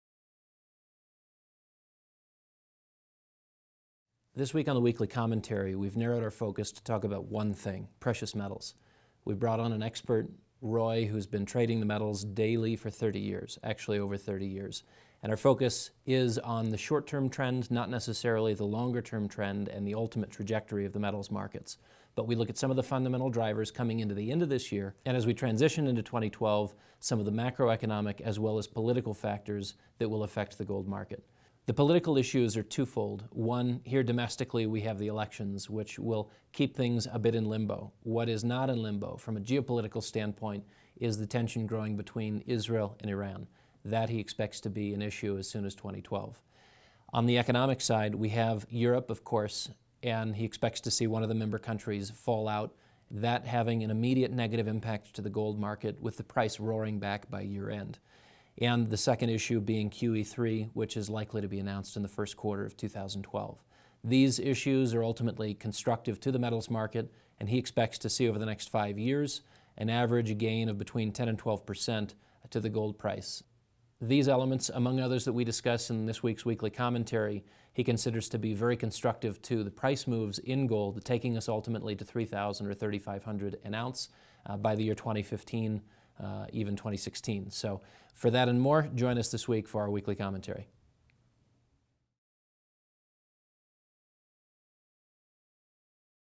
A Street View of Precious Metals: Interview with Trader Roy
-Gold should see $1850 in the next 60 days and should be very bullish in 2012 as well.
-Two macroeconomic events that will drive gold over the next year are loose monetary policy and the potential collapse of an EU member nation.
-Gold should maintain steady double digit growth through at least 2015. Growth could be much more abrupt should something unexpected occur such as an Israeli strike on Iranian nuclear facilities or a surprise in the U.S. Presidential election.